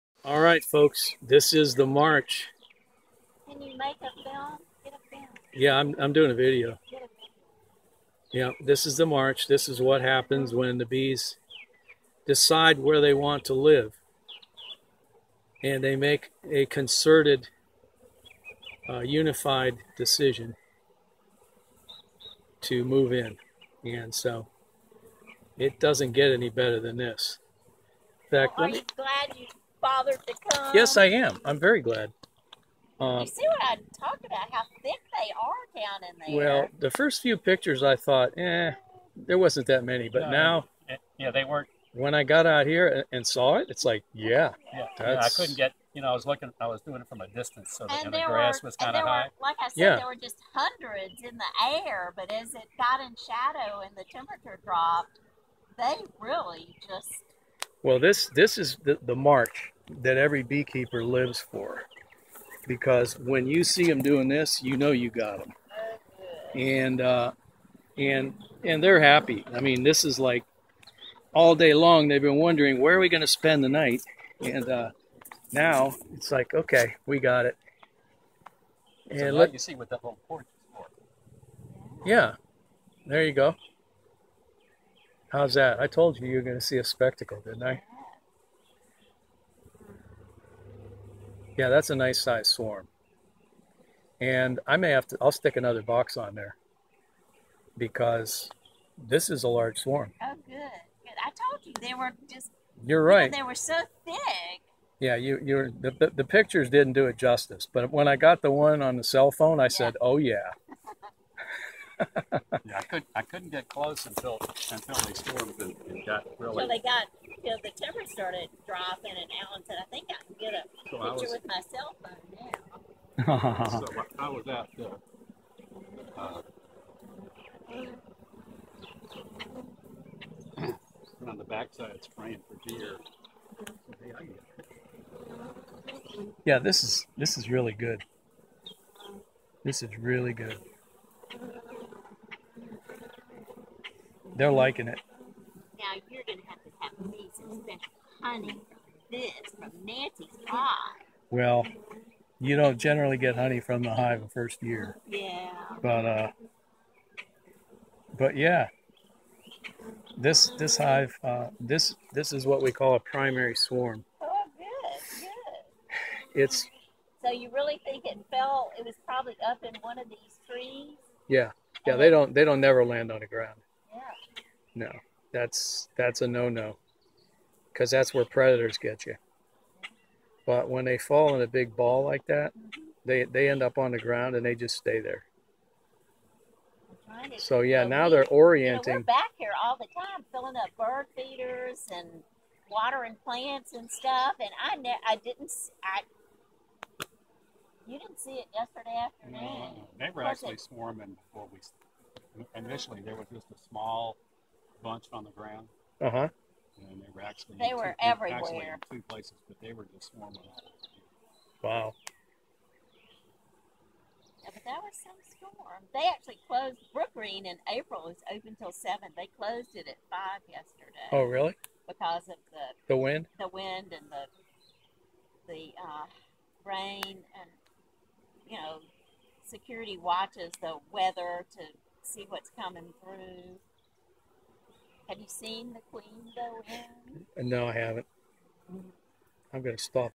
The March
I received a call from a friend who said she had a large swarm of honey bees in her backyard. She was afraid to go back there for fear of the bees.

They sent me some pictures, and at first, it did not seem like a very big swarm. But, when I got to the site, I realized that it was a very large swarm of nice bees.

In the video you can see the bees marching into my hive box. It is truly a spectacle to behold when they begin to march in a very orderly fashion. This was a fun catch!